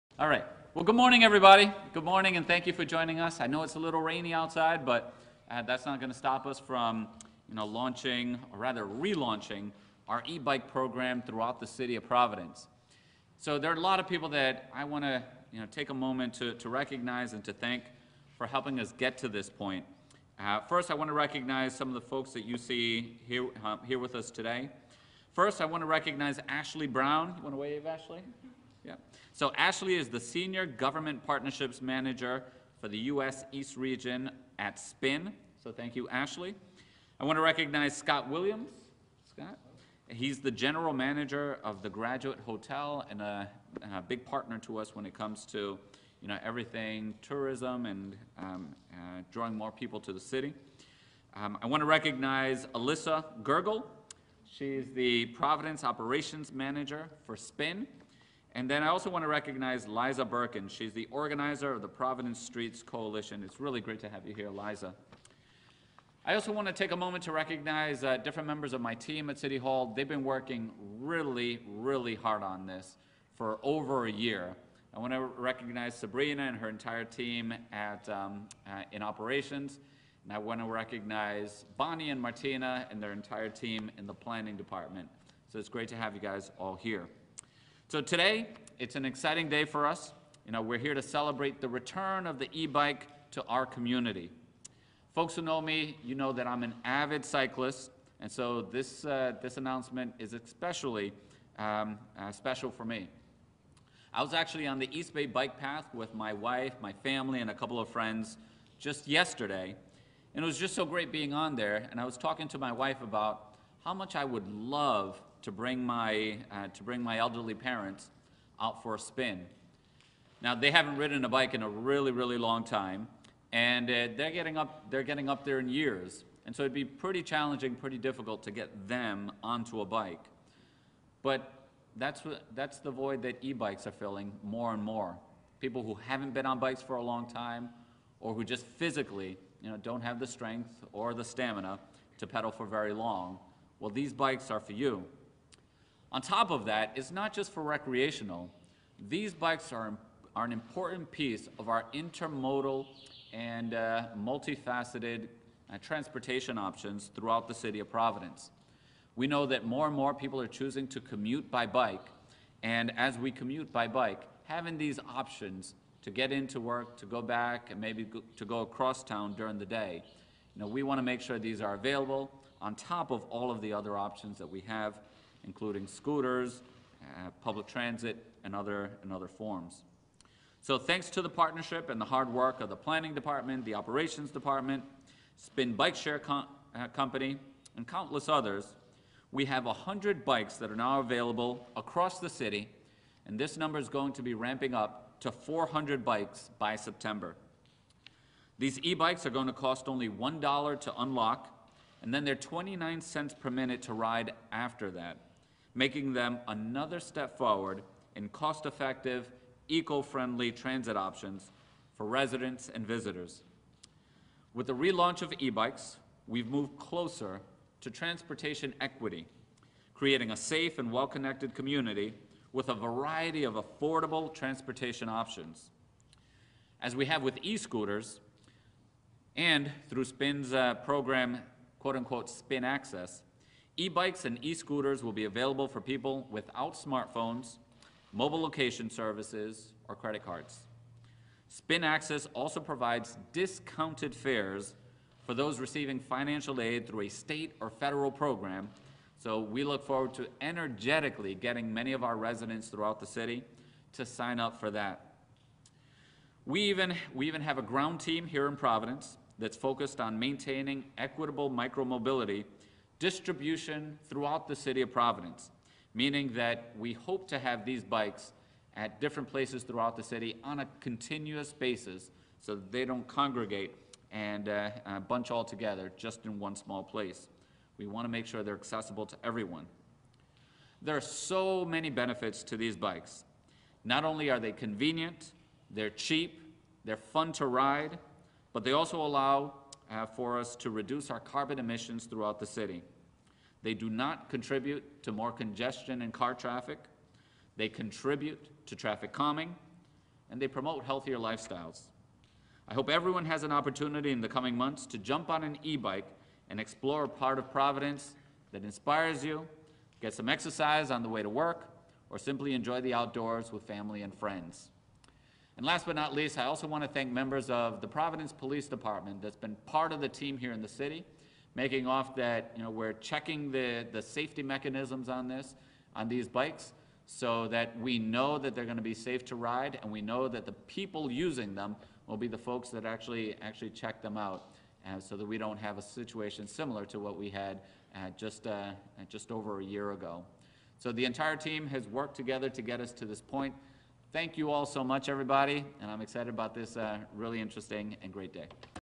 VIDEO NOW: Mayor Elorza announces the relaunch of an electric bike share program in Providence
Mayor Elorza announces the relaunch of an electric bike share program in Providence on Monday, June 14.